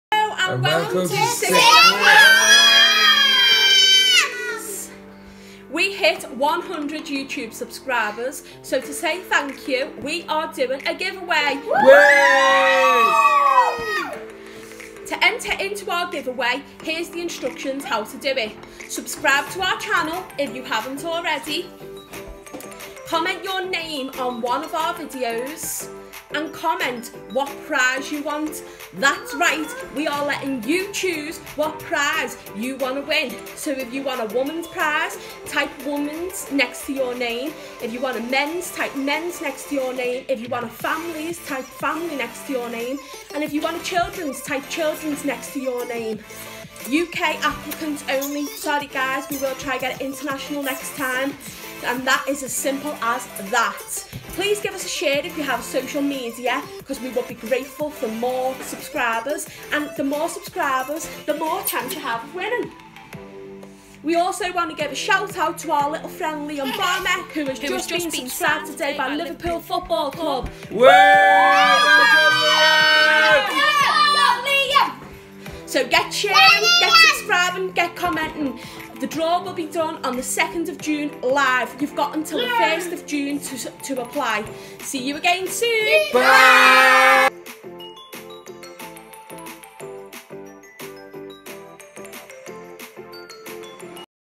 ****now ended*** How to enter for our 100 subscribers give away
How to enter for our 100 subscribers give away uk entries only this time sorry guys !!!

•comment your prize choice
That’s right we are letting you choose what type of prize you would like
•women’s
•men’s
•family’s
•children

Last entry 1st June 2018
Live draw 2nd June 2018